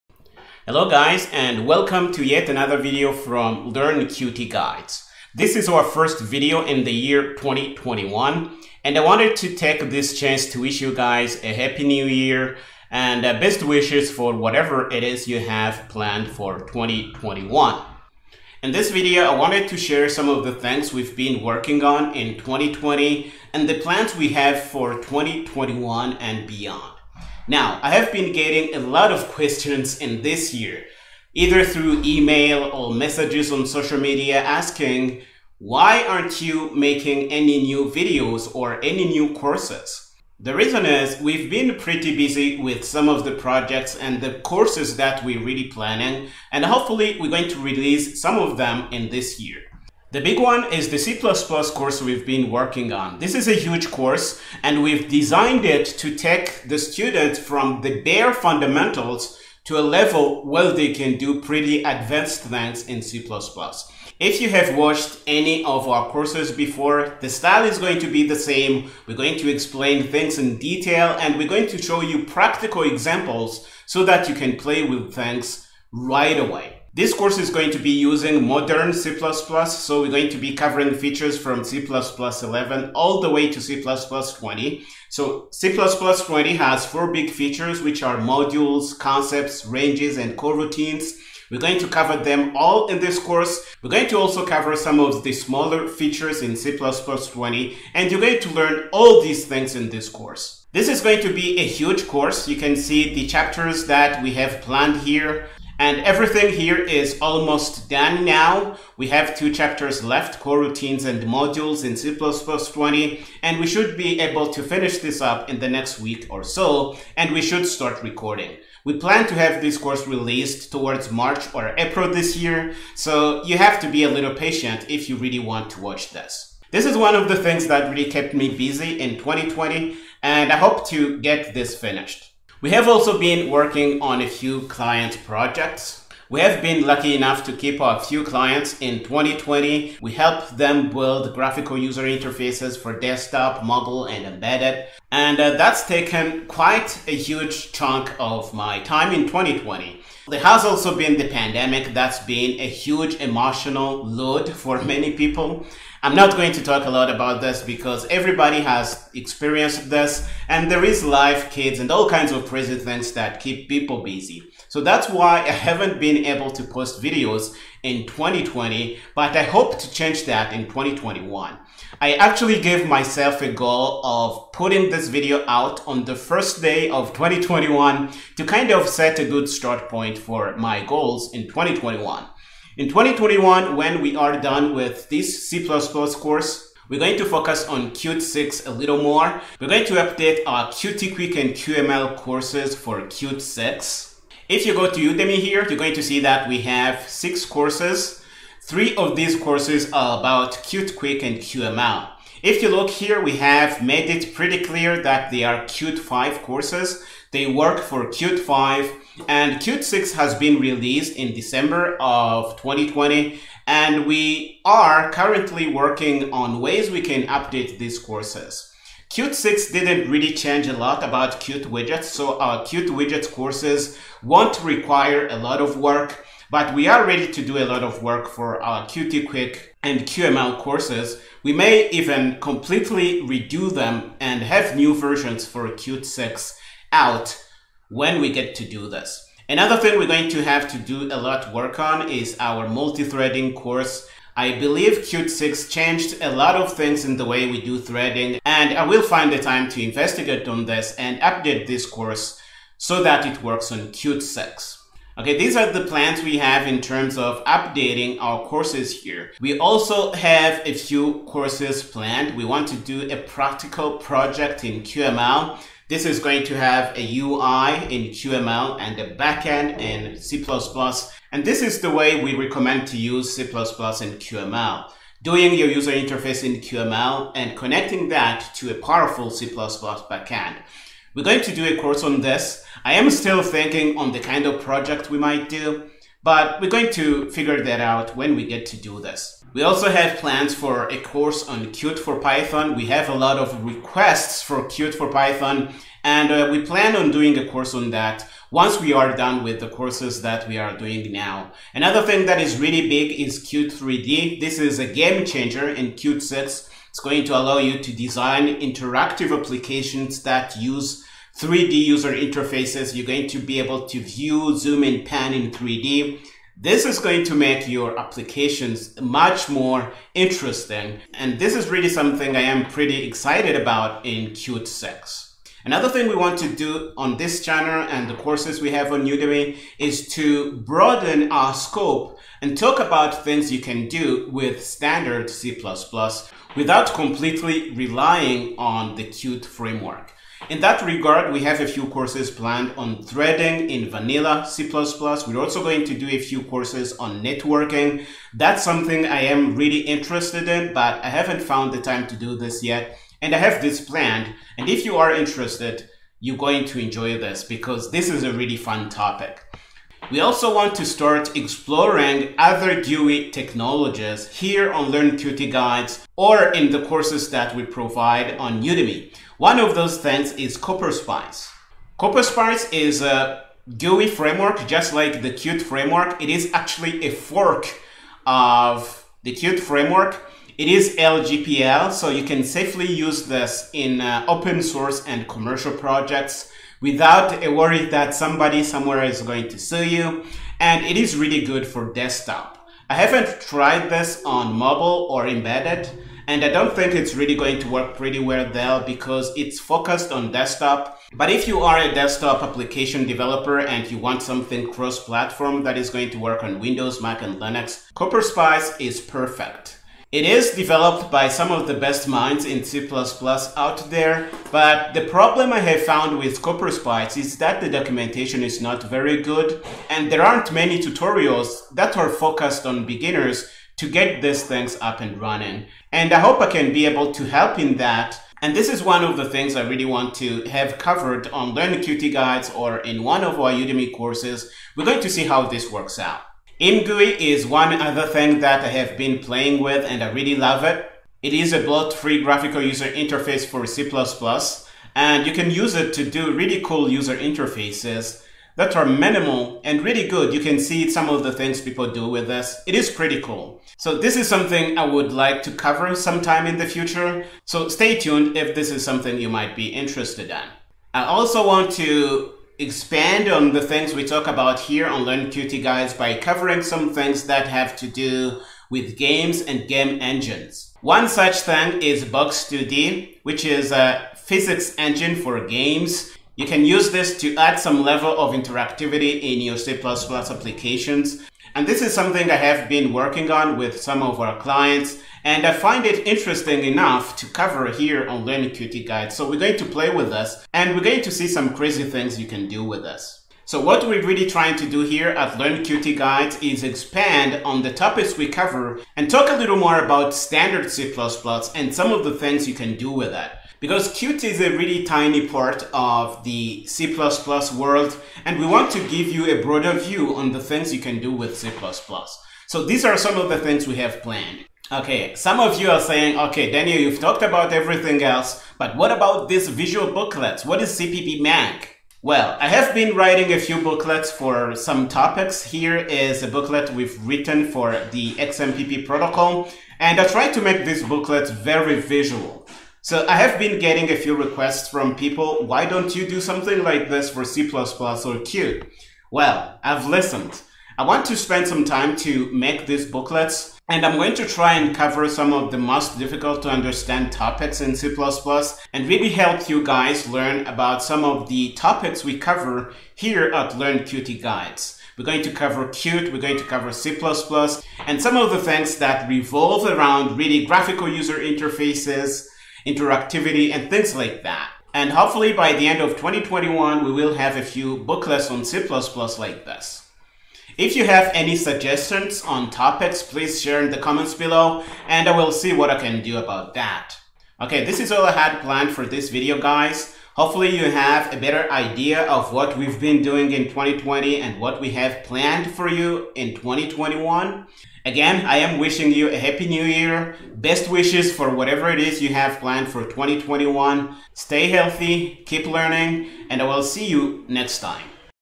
Happy New year - What's Daniel(LearnQtGuides) been up to in 2020 - Plans for 2021 for Qt/C++/QML.
LearnQtGuides is wishing you guys a happy start for 2021. Shading some light on what's been holding me from releasing new courses and videos in 2020 and some exciting new things coming up in 2021 in terms of content on Qt,C++ and QML.